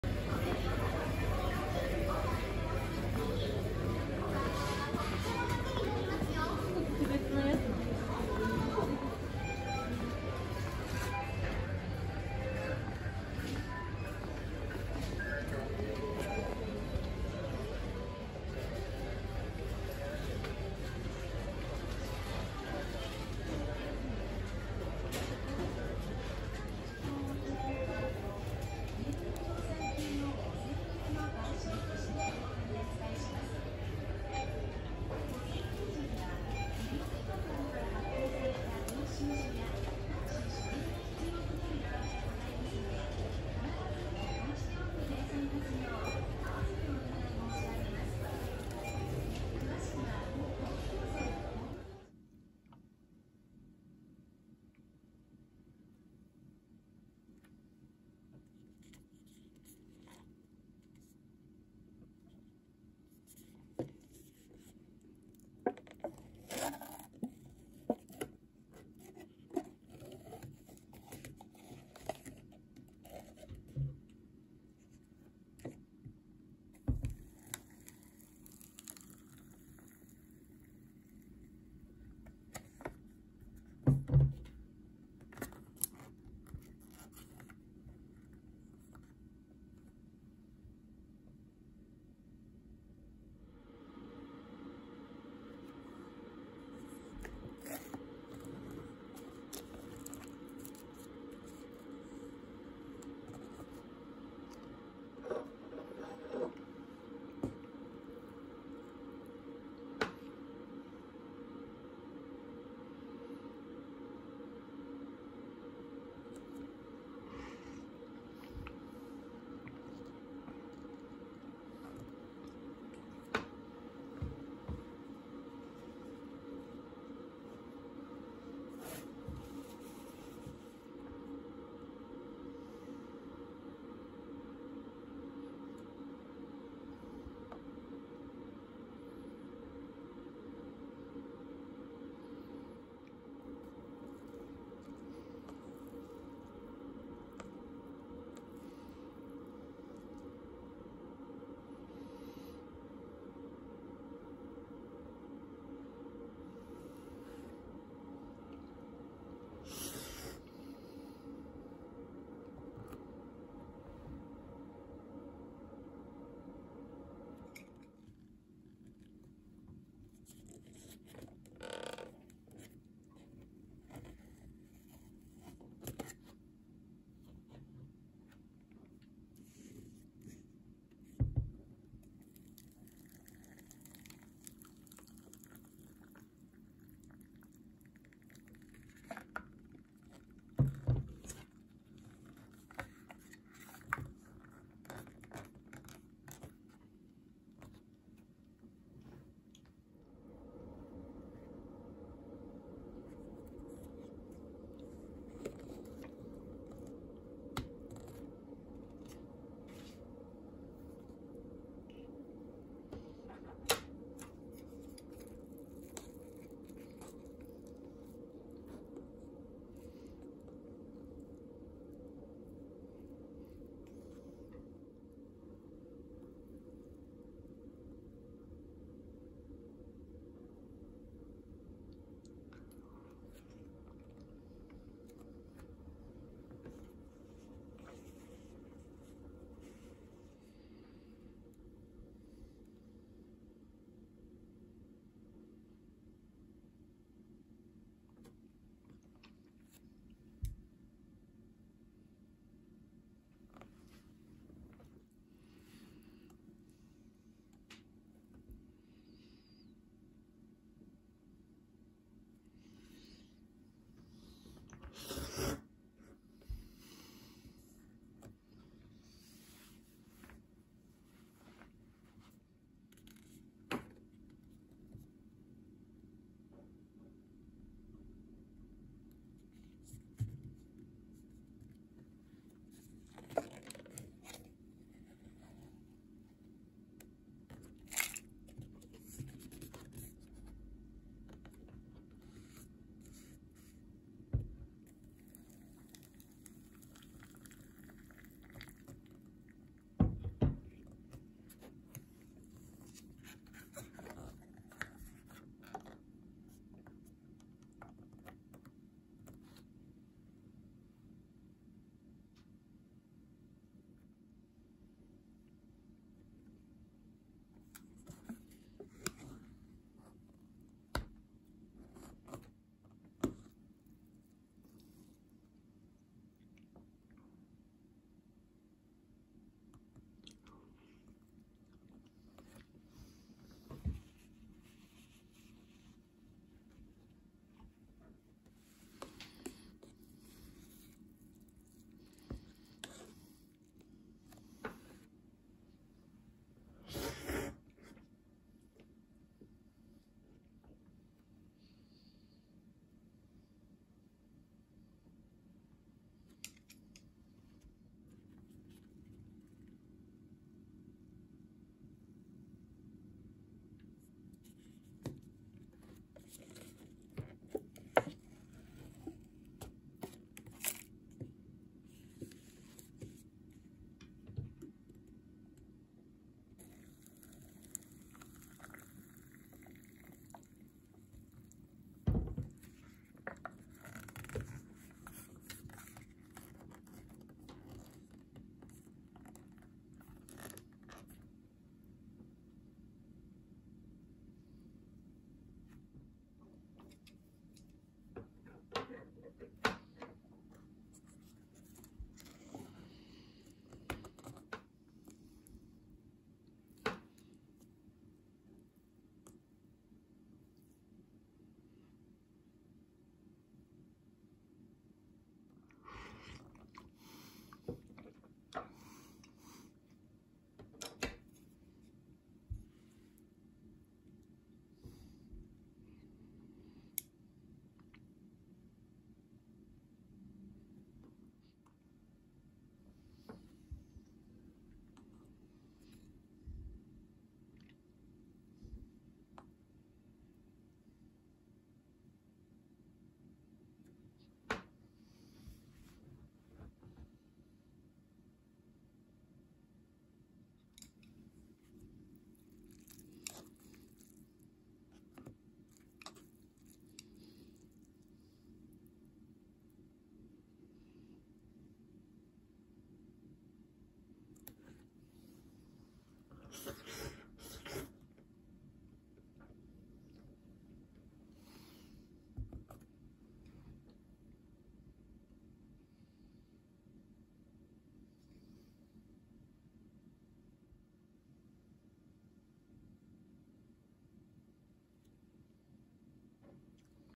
Instant Ramen for Kids
I would like to introduce instant ramen, which has a long history in Japan.
Its name is 'Cup Noodle'.

This time I will introduce a smaller size of it.
Even children can easily eat it up.